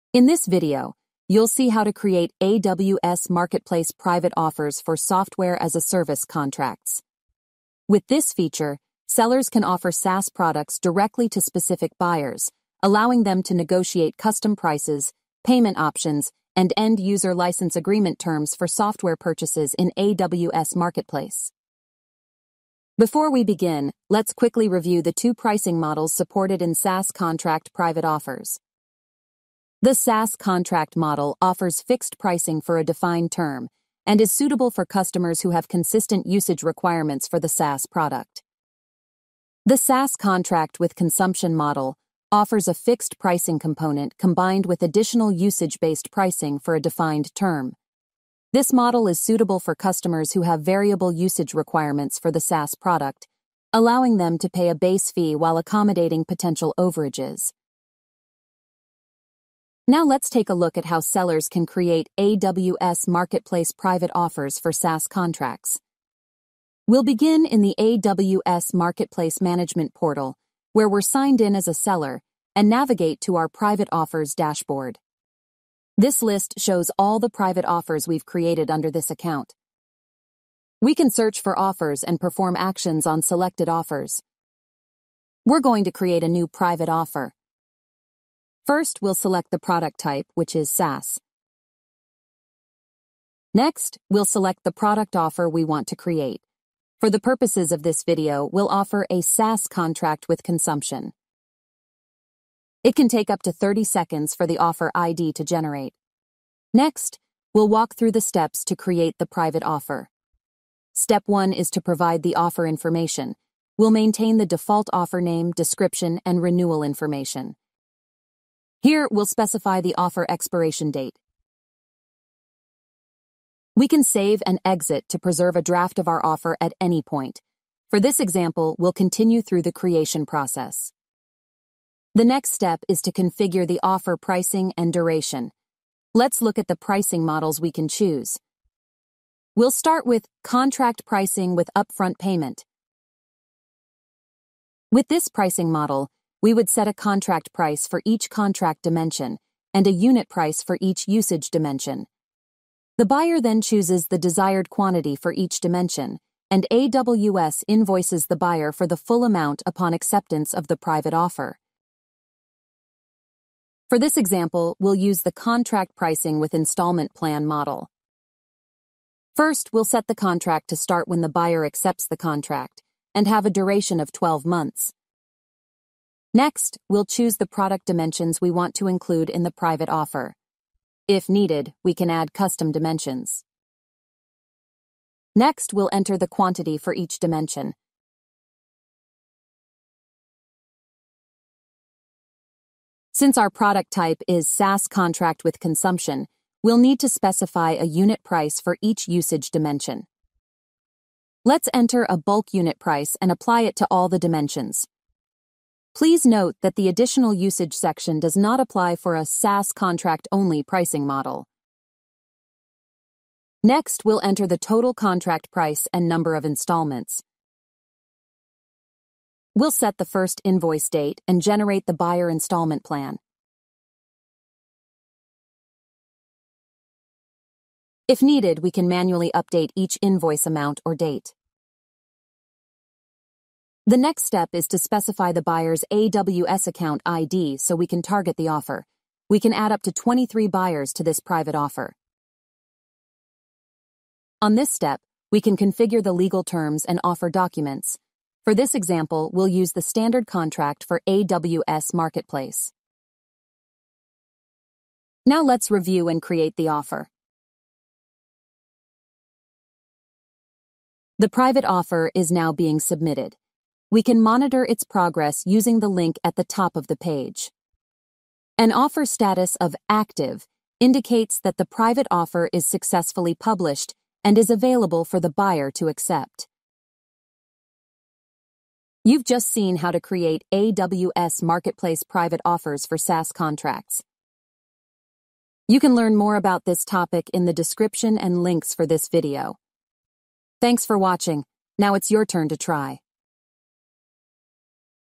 Create AWS Marketplace Private Offers for SaaS Contracts | Amazon Web Services
In this video, you'll see how to create AWS Marketplace private offers for Software as a Service (SaaS) contracts.

With this feature, sellers can offer SaaS products directly to specific buyers, allowing them to negotiate custom prices, payment options, and end user license agreement (EULA) terms for software purchases in AWS Marketplace.